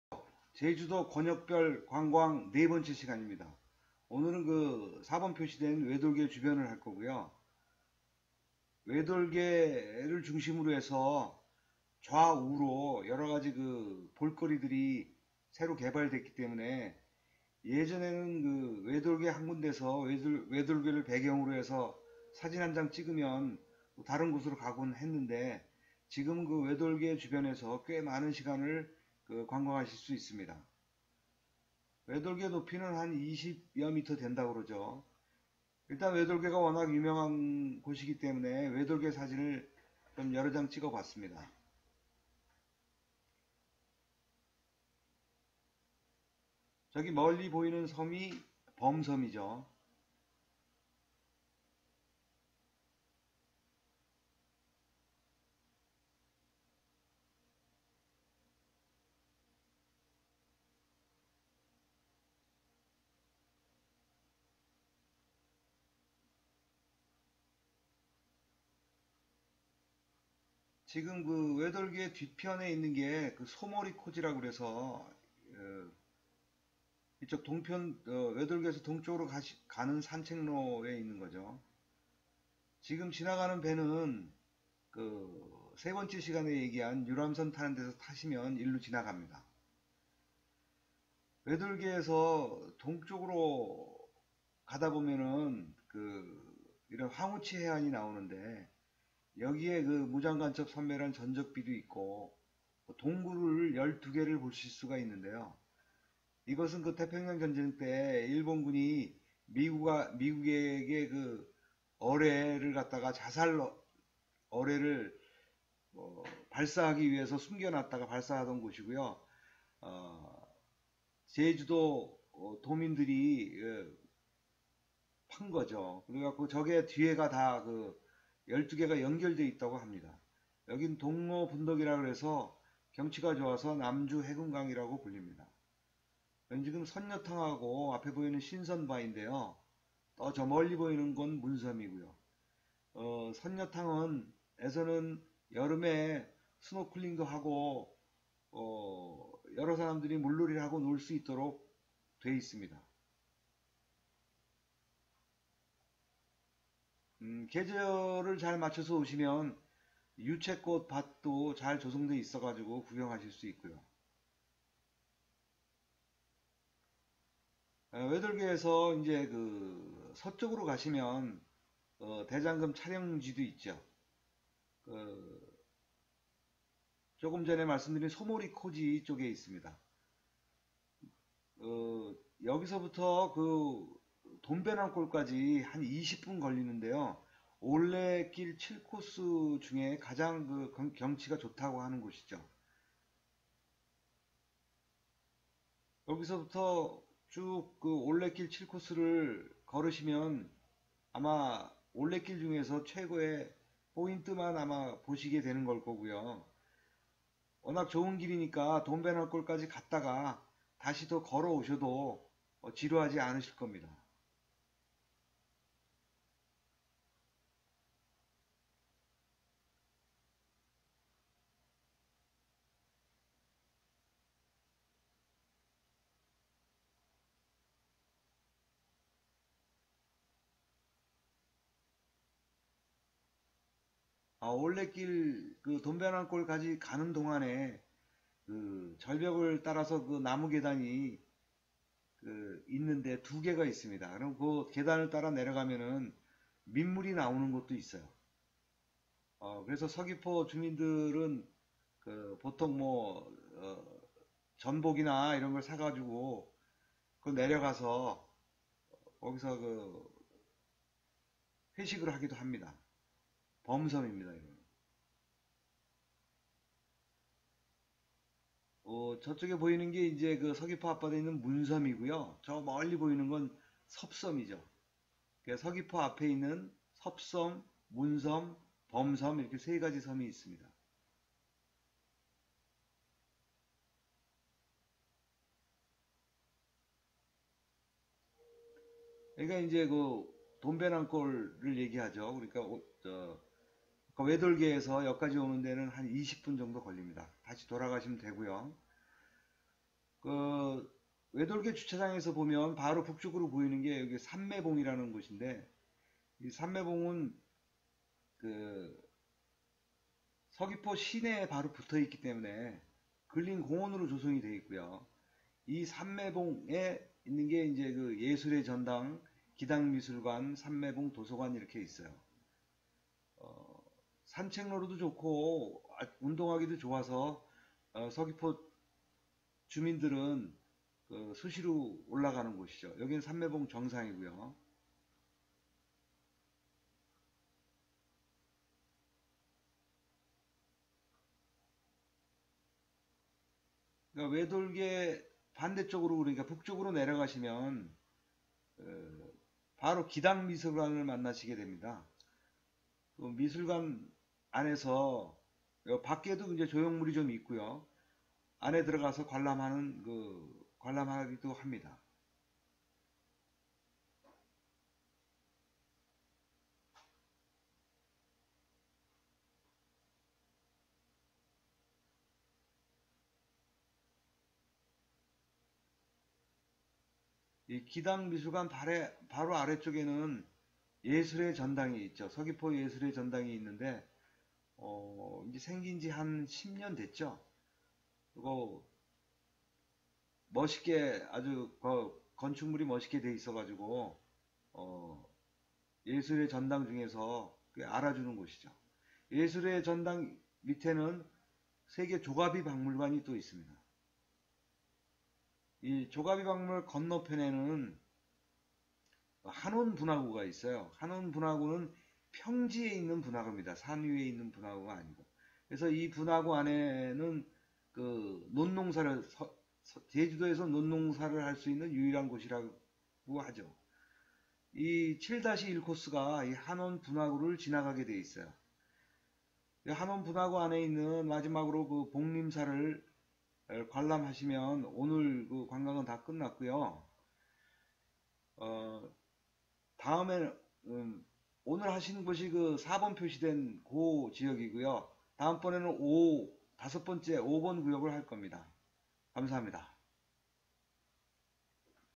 제주도 권역별 관광 여행 네번째..외돌개,황우치해안,선녀탕,신선바위,올레 7코스산책로,삼매봉,돔베낭골,예술의전당,세계조가비박물관,기당미술관,하논분화구,봉림사
제주도 전체를 33개구역으로 나눠서 구역별로 관광할수있도록 관광지를 안내...
관광지명과 대표사진으로. 자세한 설명은 지명으로 검색하면 제주도관광의 완성..